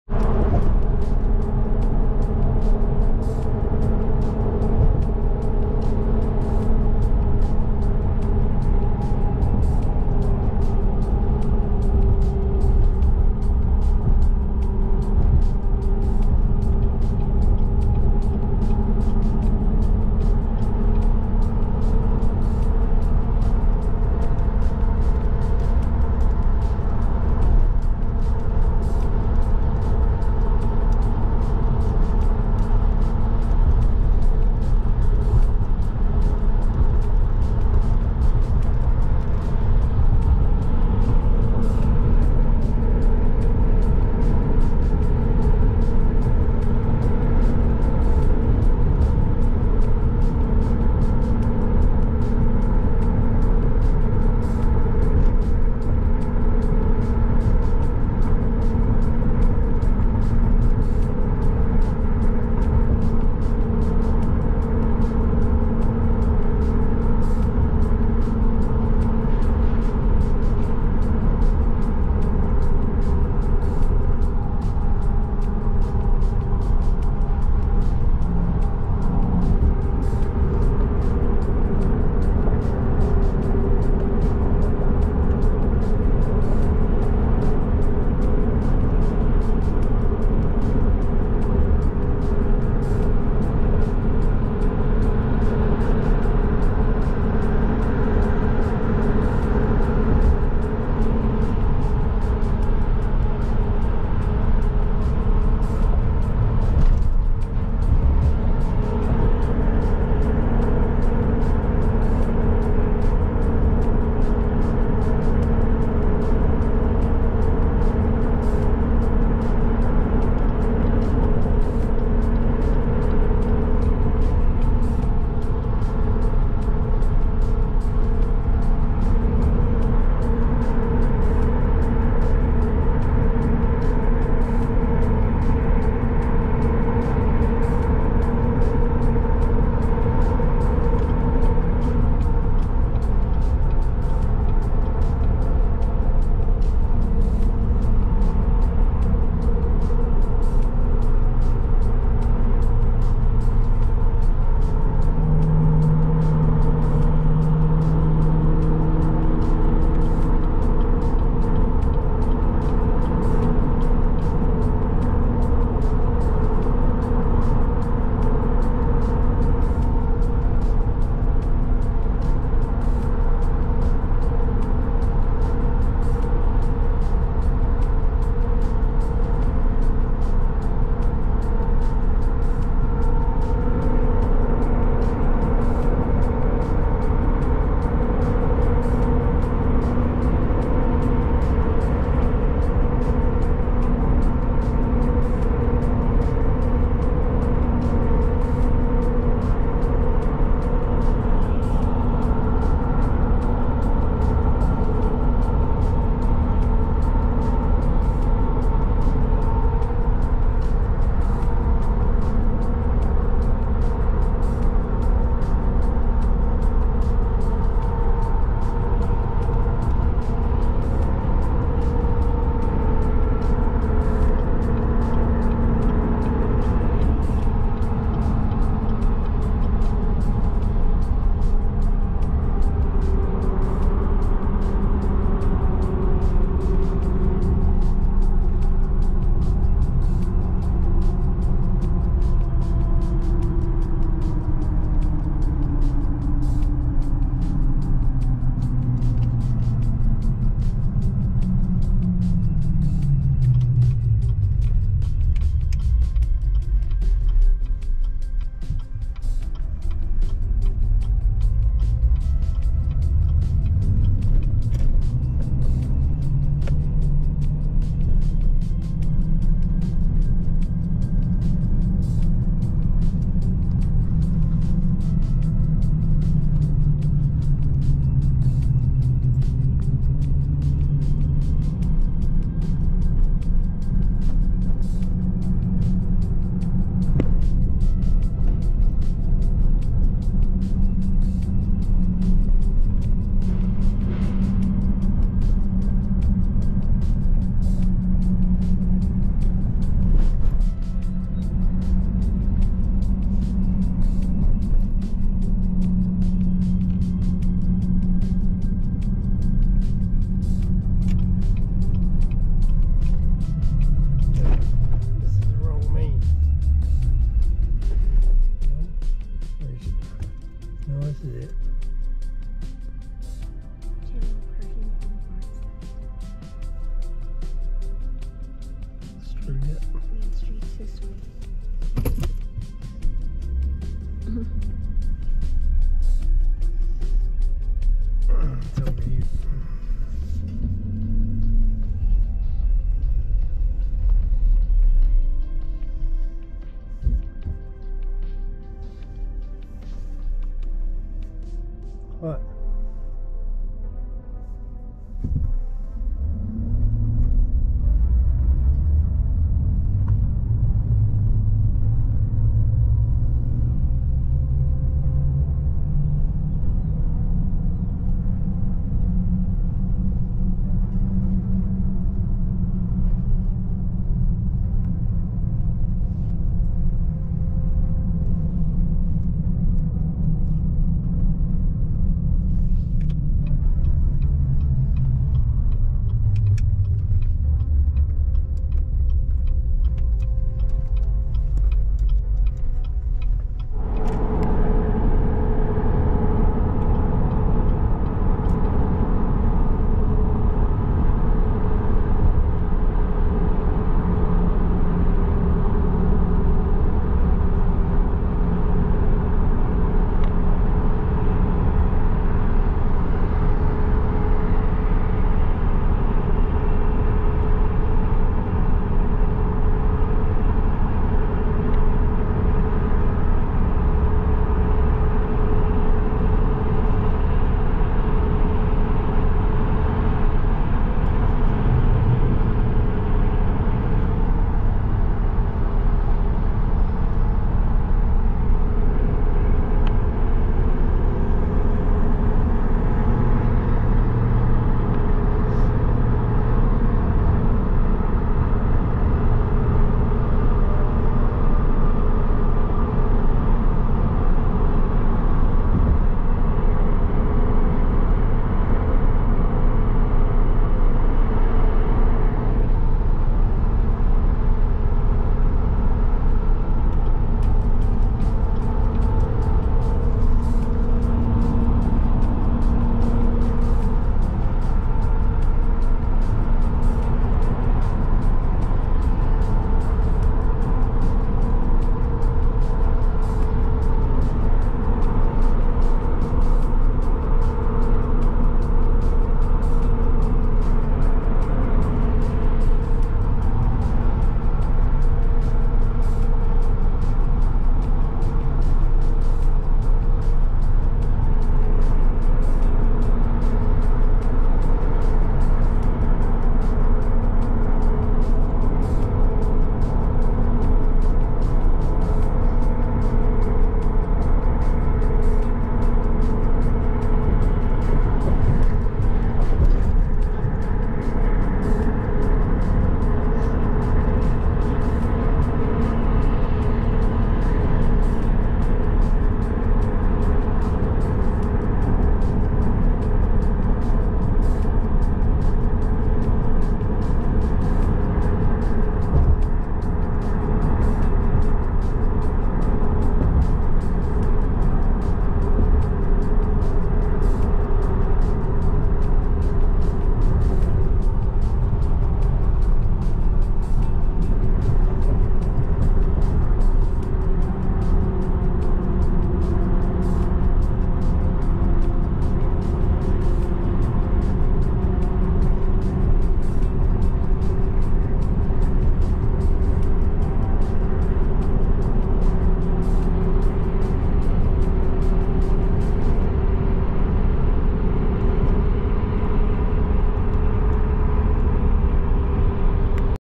Oklahoma City And I Filmed The Homeless
We had to take sheet rock and supplies back to OKC and I wanted to share a short ride to downtown OKC and I was shocked at all of the homeless we saw and all of the trash. I have never seen so many homeless. Very concerning.
We need to do something about our country!!!